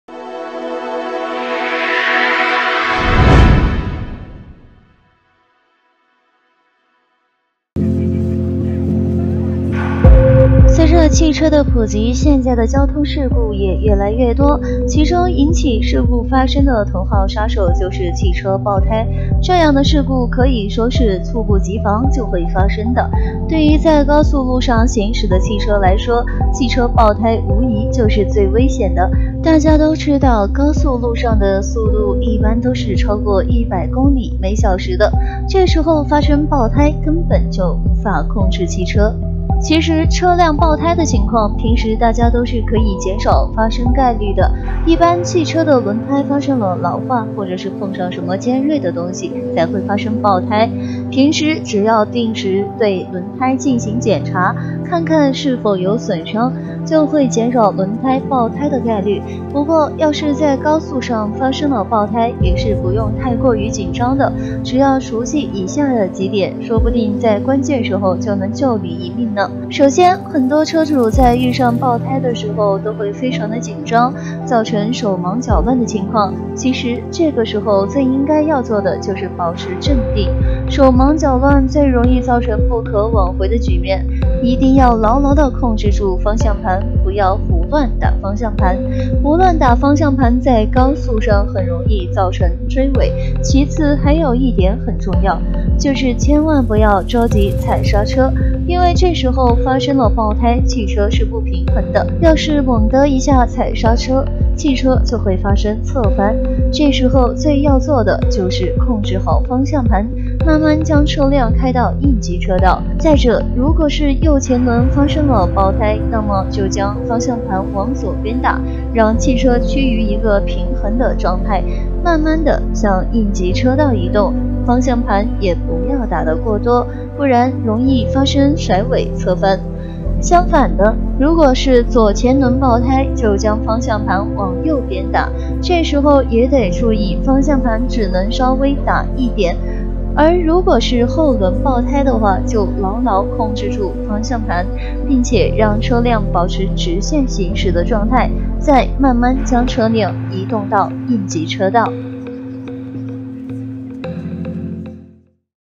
在高速上發生爆胎怎麽辦？保持鎮定，記住這三點關鍵時刻能救命
隨著汽車的普及，現在的交通事故也越來越多，其中引起事故發生的頭號殺手就是汽車爆胎，這樣的事故可以說是猝不及防就會發生的，對於在高速路上行駛的汽車來說，汽車爆胎無疑就是最危險的，大家都知道高速路上的速度一般都是超過100km/小時的，這時候發生爆胎，根本就無法控制汽車。